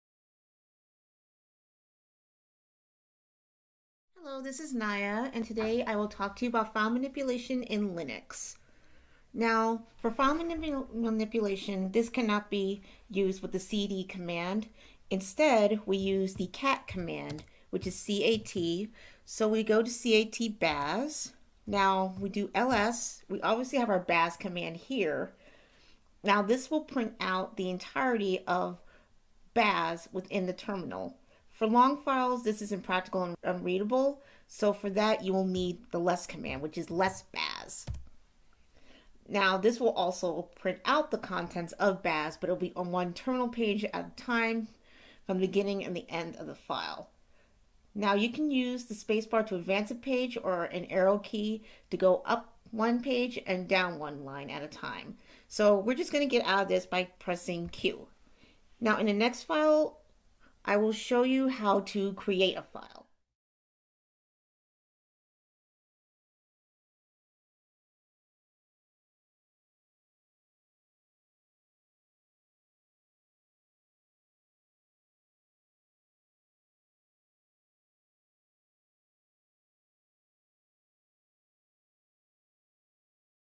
Intro to Linux Part 5: File Manipulation
The video shows how to do file manipulation in Linux.